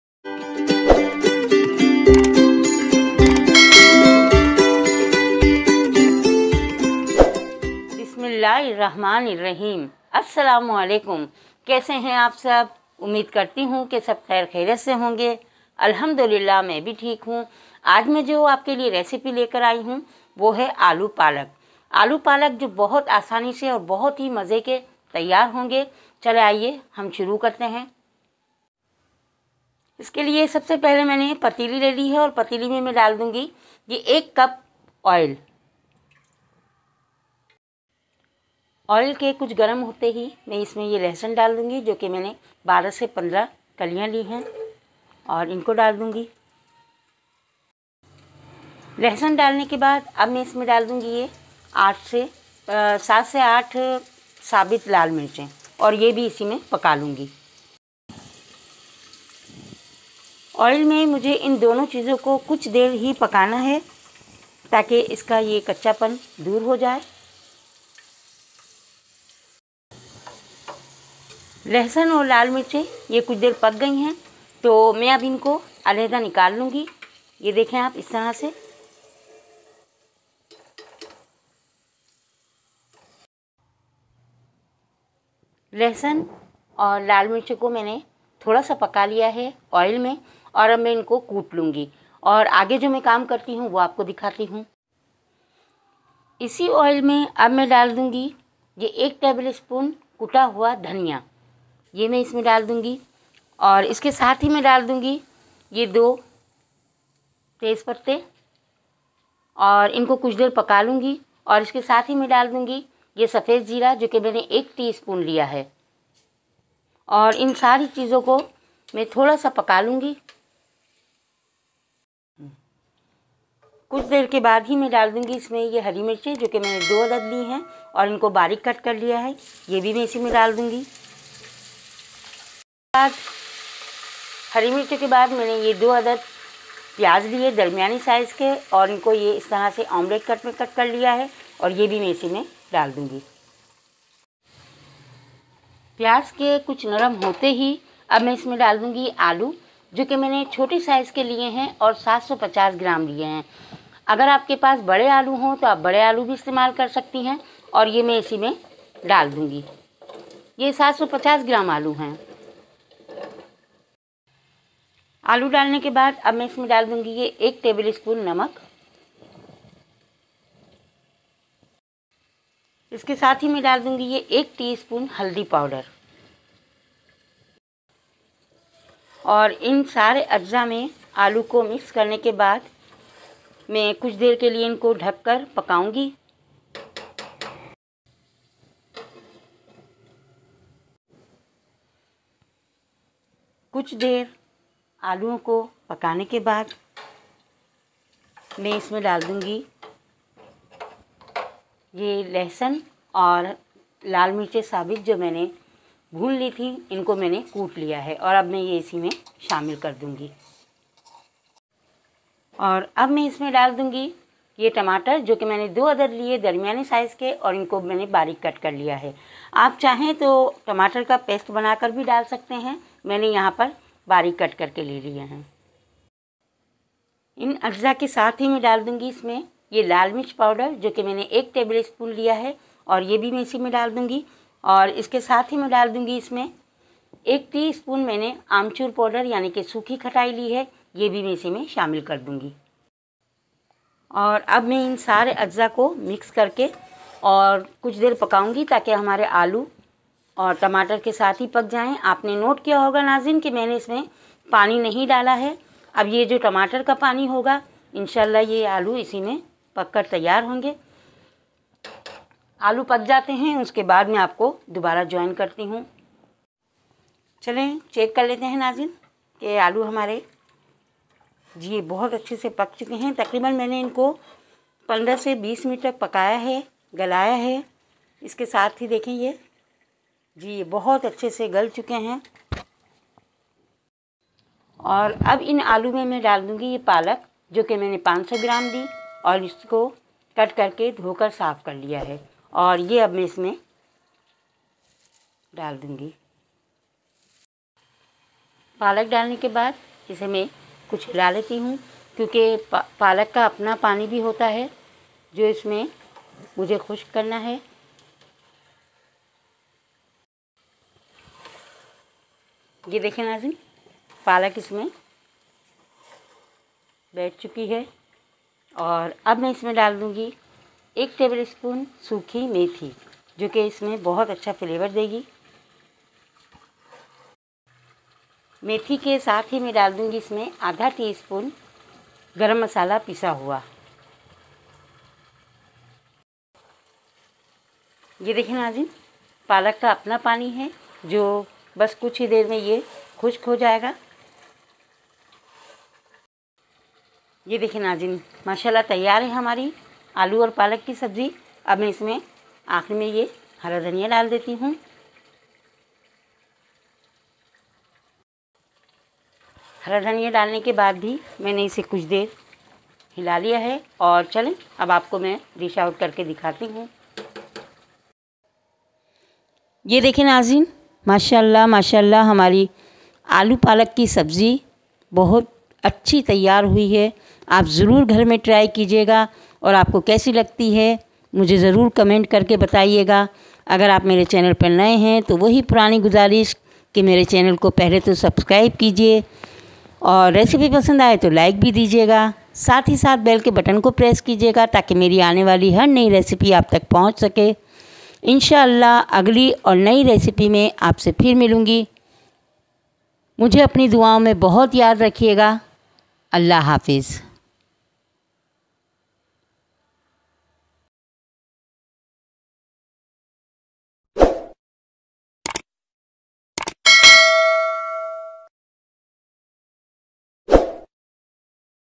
Aloo Palak Ke Sabze#Cooking With Halima#۔۔۔آلو پالک کی سبزی
ghar ke masalo se tayyar Ke hoe Aloo Palak Ke sabze zaike or lazzat ke sath.